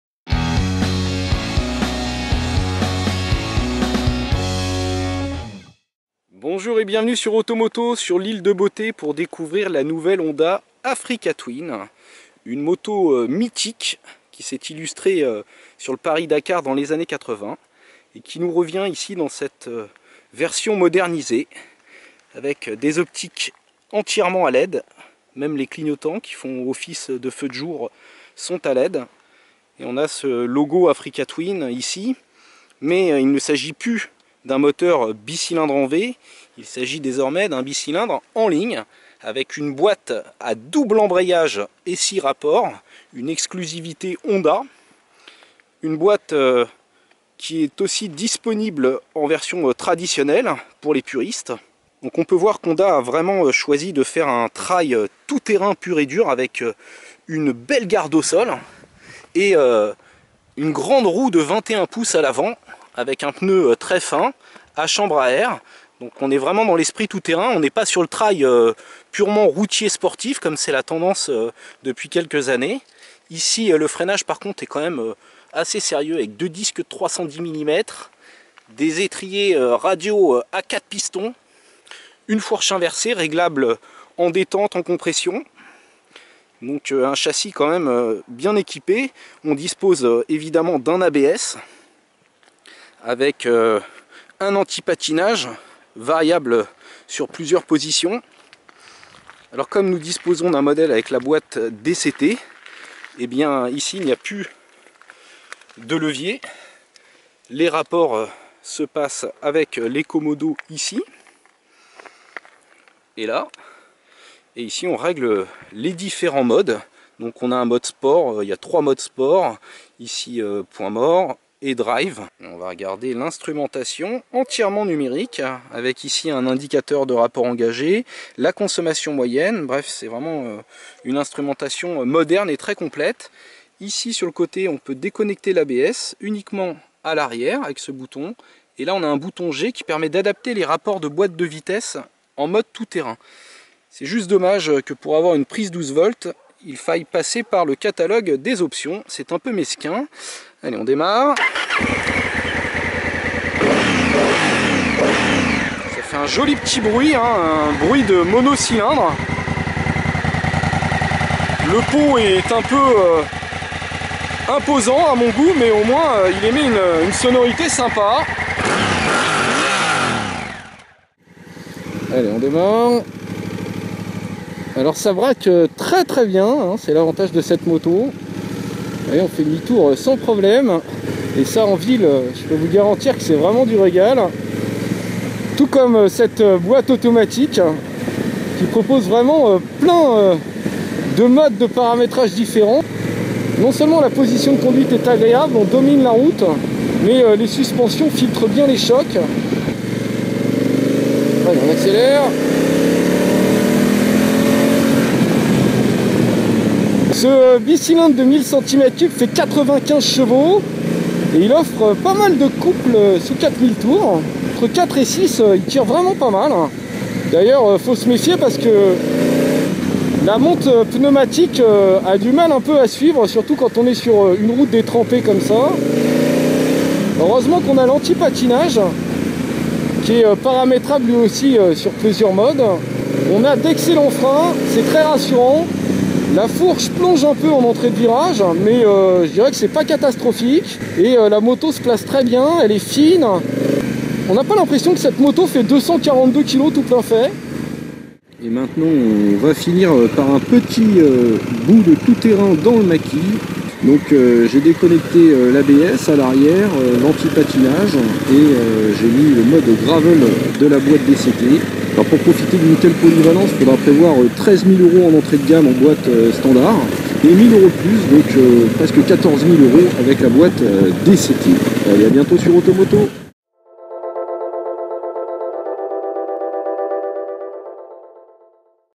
2016 Honda Africa Twin 1000 CRF [ESSAI] : parée pour le pire - Auto Moto Magazine
NOUVELLE AFRICA TWIN 2016 - La nouvelle génération de la Honda Africa Twin tente de séduire aussi bien les amateurs de tout terrain que les urbains. Un essai réalisé par Maxime Fontanier.